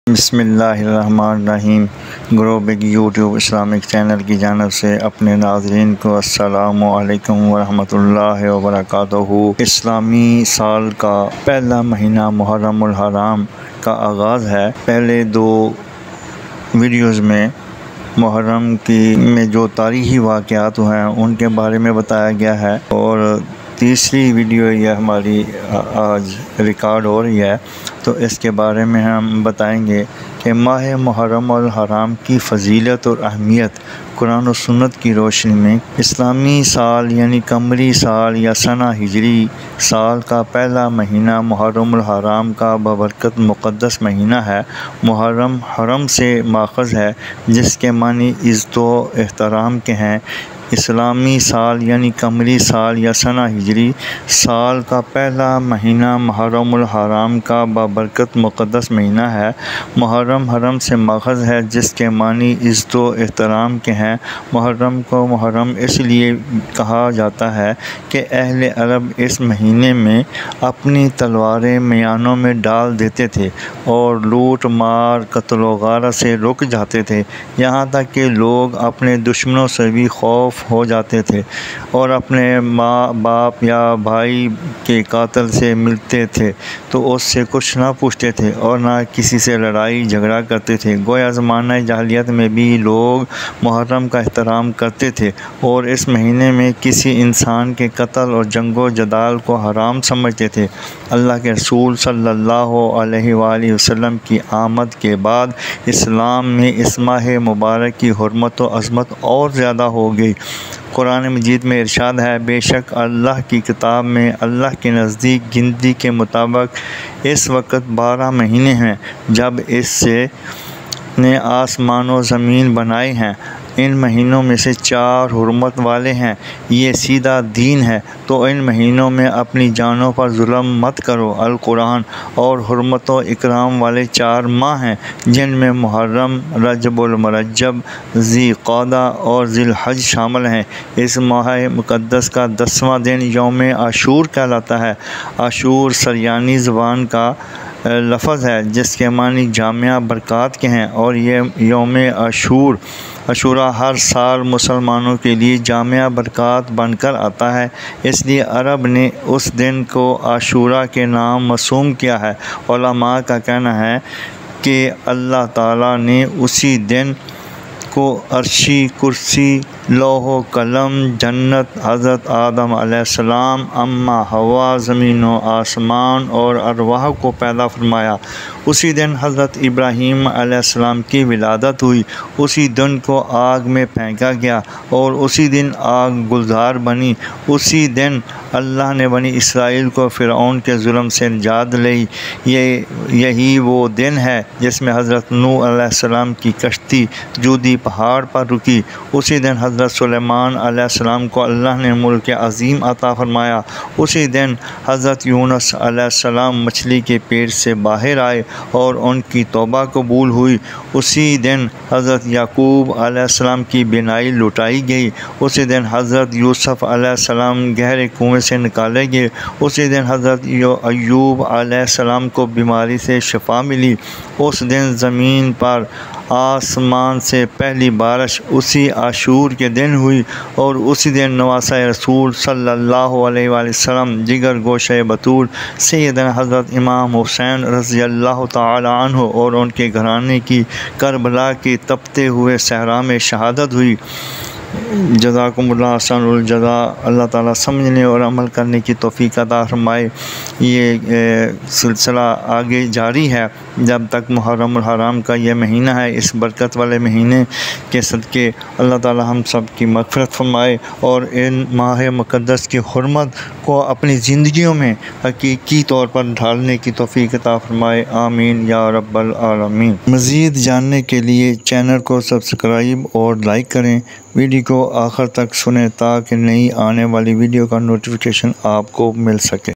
Muharram Ul Haram ki Ahmiyat aor Fazeelat ||ماہ محرم الحرام کی فضیلت واہمیت
HI
اسلامی سال کاپہلا مہینہ
پارٹ-1
قمری سال
محرم الحرام
کابابرکت مقدس مہینہ ہے
عزت واحترام
اپنی تلواریں میانوں میں ڈال دیتے
لوٹ مارقتل وغارت سے رک جاتے
قتل اور جنگ وجدال کوحرام سمجھتے
اللہ کے رسول اللہ صلی اللہ کی آمد کے بعد
بعد اسلام میں اس ماہ مبارک کی حرمت وعظمت اور زیادہ ہوگئی

if you like Grow Big Videos so must click on Subsucribe button for more interesting videos